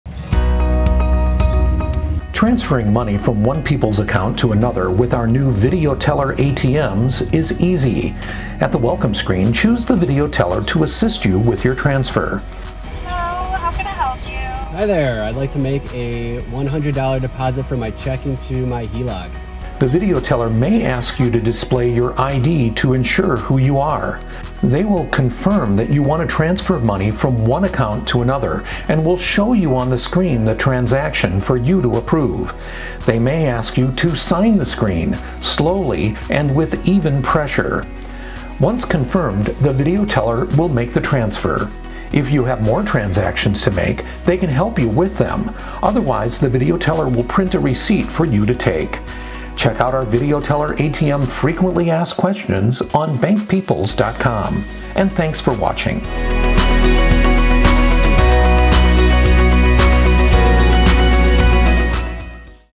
Video Teller ATM Transfers
How to transfer money from one Peoples account to another using the Video Teller ATM